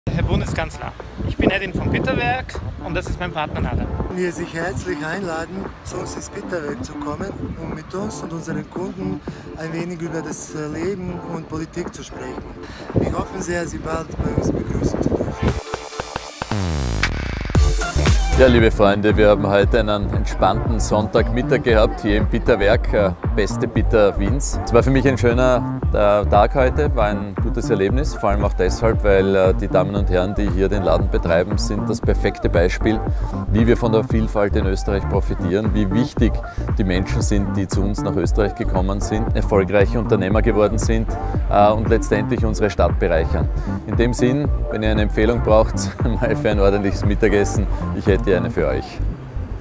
Christian Kern im Pitawerk
Bundeskanzler Christian auf Besuch im Pittawerk in Wien Rudolfsheim-Fünfhaus